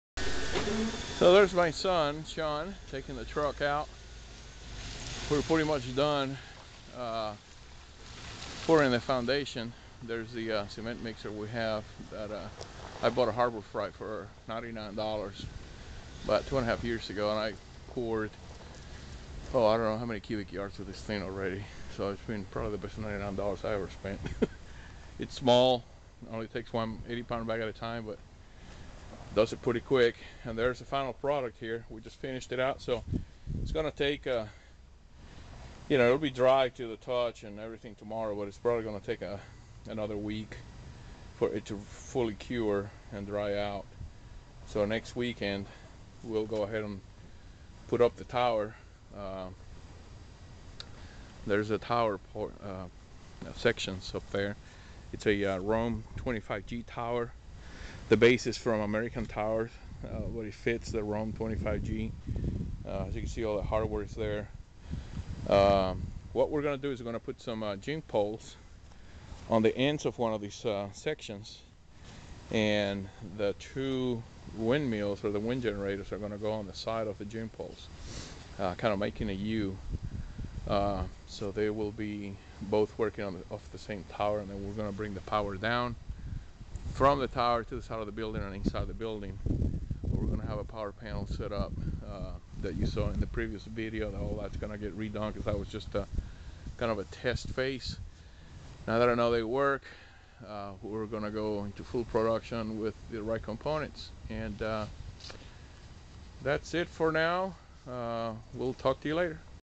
Wind turbine generator tower installation Part 2
Second part of the new wind turbine tower. It will have two WindMax 1Kw turbines on top a Rohm 25G tower with a tilt-up base.